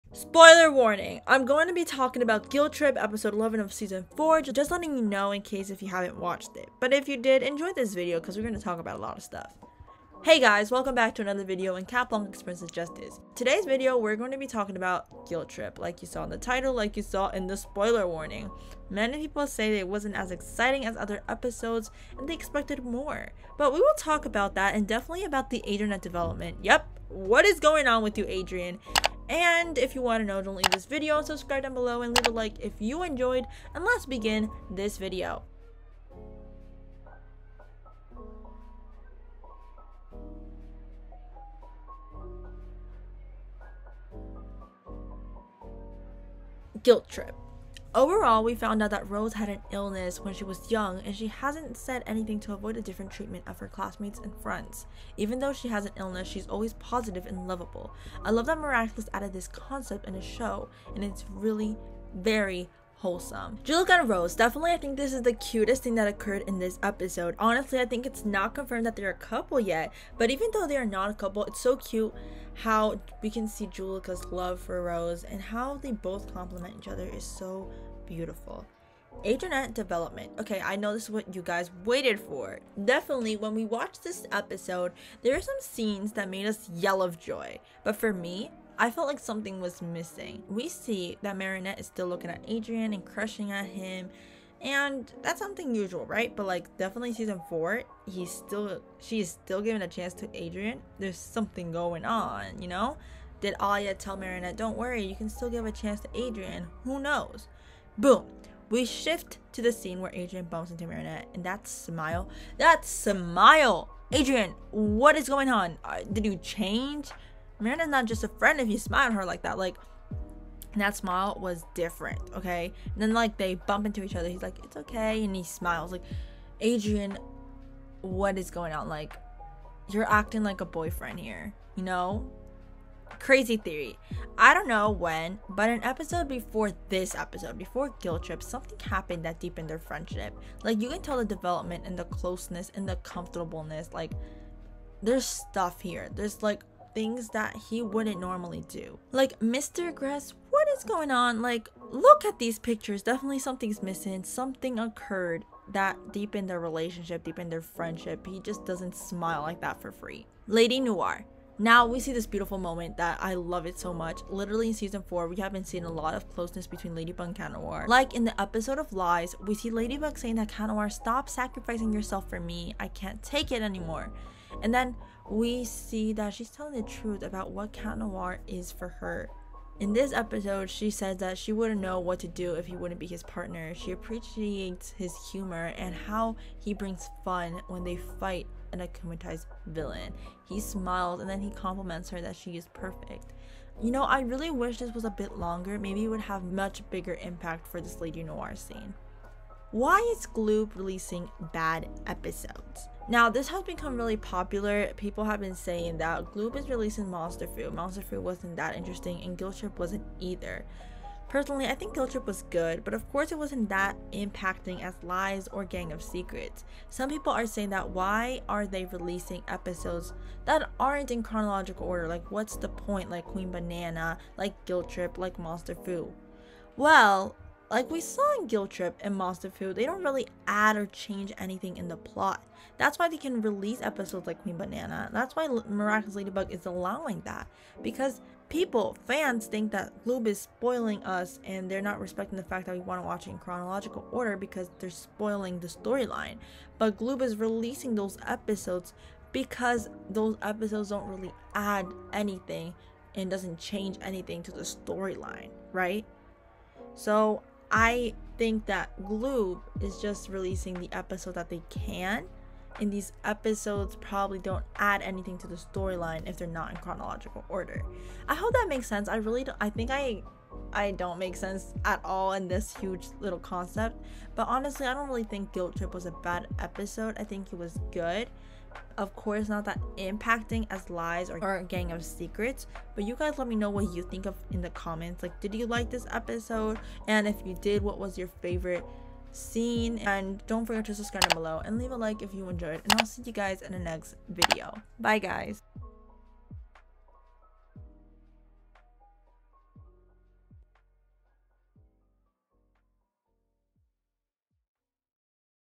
What didn't you see in GUILTRIP | Miraculous Ladybug Season 4 Analysis/Theory [Spoiler Warning]
Hey guys welcome back to another video in Cat Blanc X Princess Justice! Follow to always have the latest spoilers and theories of Miraculous Ladybug.